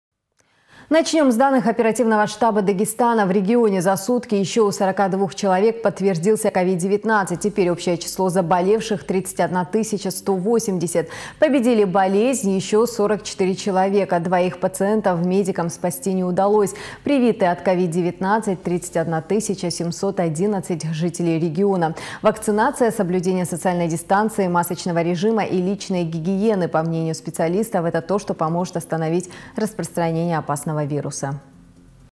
В регионе за сутки еще у 42 человек подтвердился COVID-19
Победили болезнь еще 44 человека. Двоих пациентов медикам спасти не удалось. Привиты от COVID-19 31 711 жителей региона.